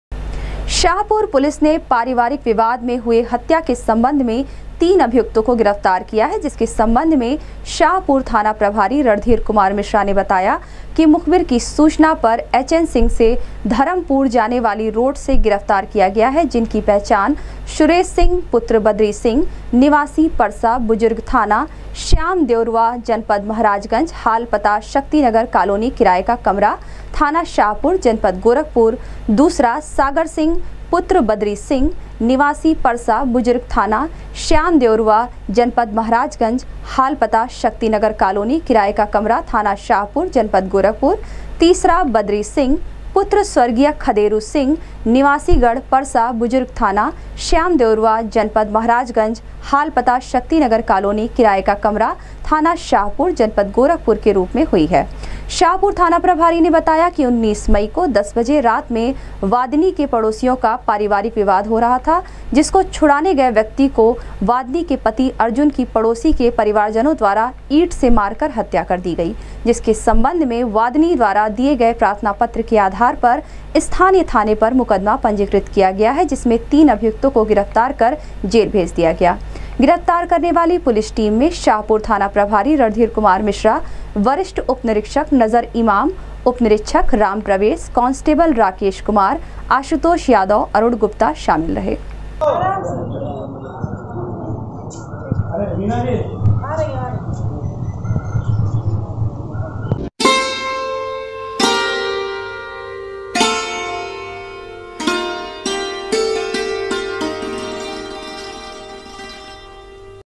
19 मई को पारिवारिक झगड़े में हुए विवाद को छुड़ाने गये व्यक्ति की हत्या करने वाले तीन अभियुक्त को
19 मई को पारिवारिक झगड़े में हुए विवाद को छुड़ाने गये व्यक्ति की हत्या करने वाले तीन अभियुक्त को शाहपुर पुलिस ने किया गिरफ्तार,भेजा जेल